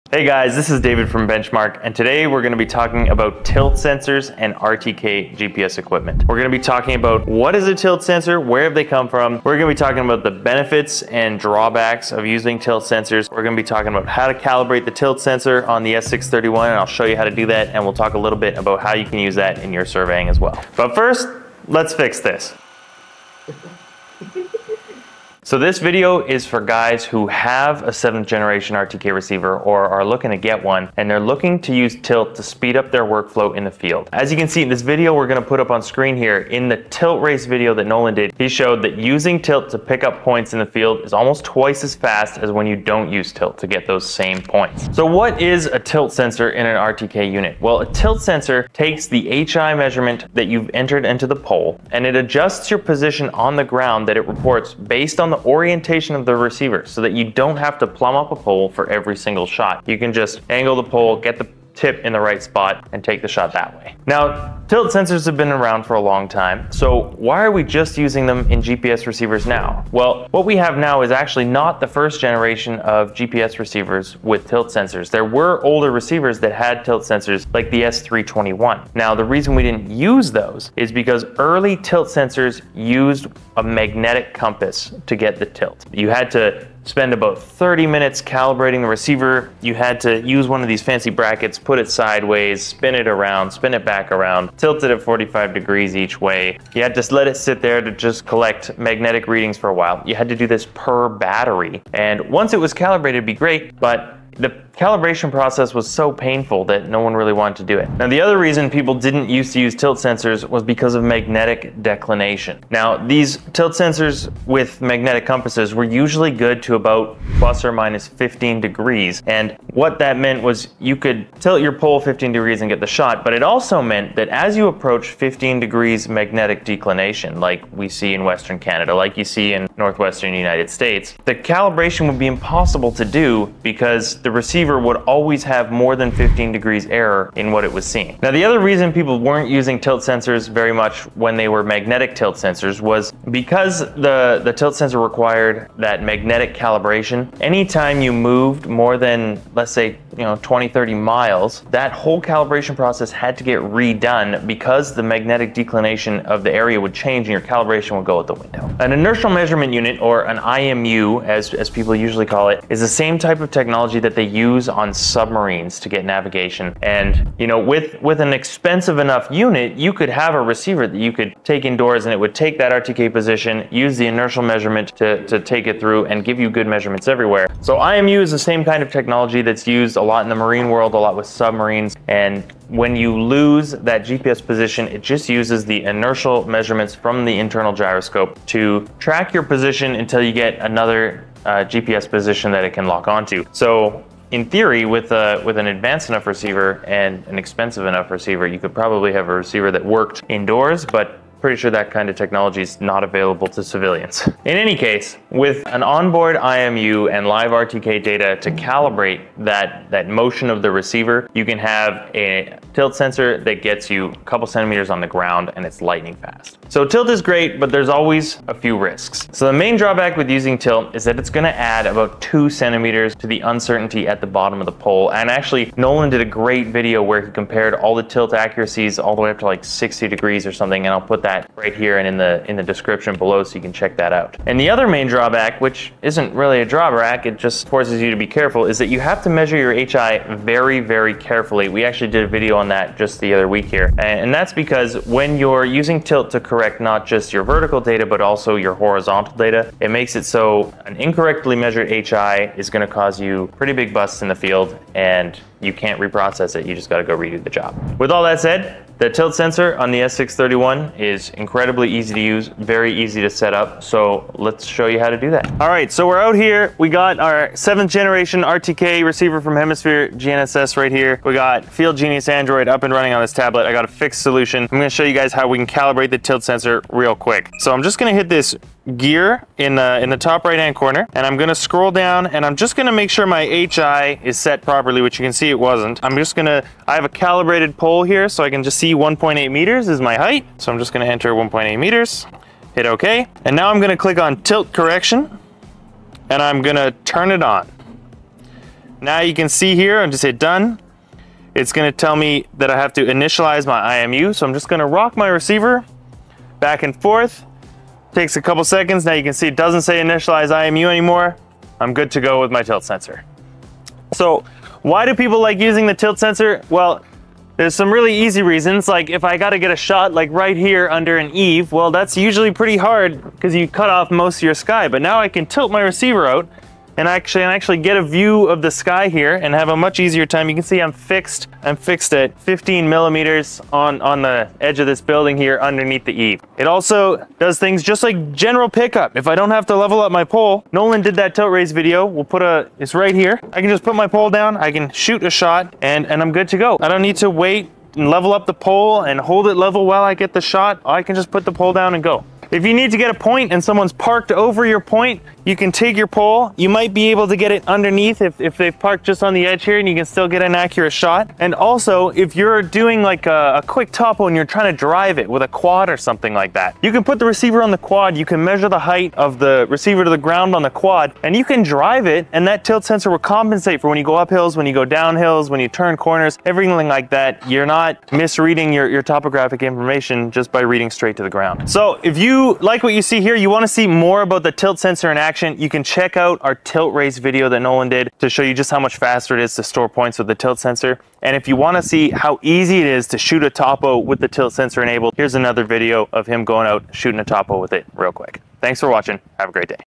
How Does a Tilt Sensor Works
Stay updated on the latest in Surveying, GNSS, and Advanced Tech

This video is a comprehensive guide on tilt sensors and their integration with the latest RTK GPS equipment. Perfect for those using a seventh-generation RTK receiver or those considering an upgrade, this video is packed with everything you need to know about tilt sensors: their history, benefits, drawbacks, and calibration processes.

What You'll Learn:
- Understanding Tilt Sensors: Discover what tilt sensors are and how they have evolved over the years.
- Calibration Guide: Step-by-step tutorial on calibrating the tilt sensor on the S631.
- Practical Applications: Insights into using tilt sensors to enhance surveying efficiency in the field.
- Benefits of using tilt in speeding up field workflows.
- In-depth comparison of tilt sensor accuracy.